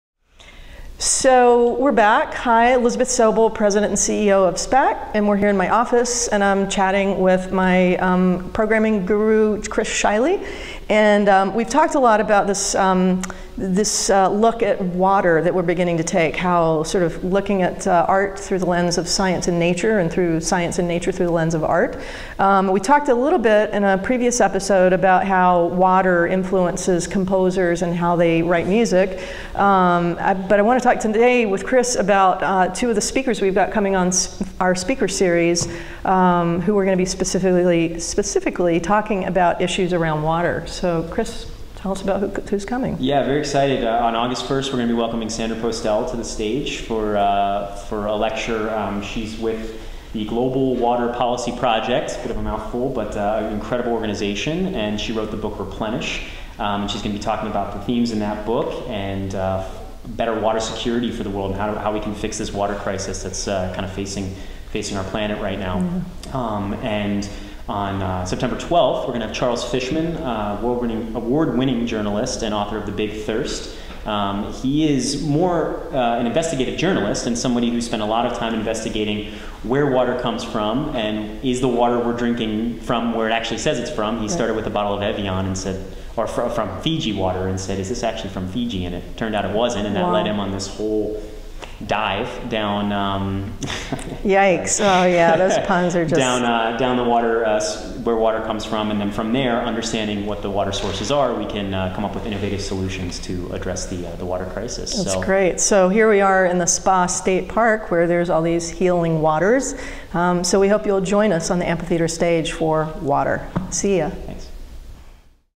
Elizabeth Sobol & Chris Shiley | SPAC's Speaker Series
Elizabeth & Chris are BACK! Today they chat about our Speaker Series which explores how water has influenced artists, nature and the environment with Sandra Postel (August 1) from Leonardo DiCaprio’s “The 11 Hour” and New York Times bestselling author Charles Fishmen (September 12).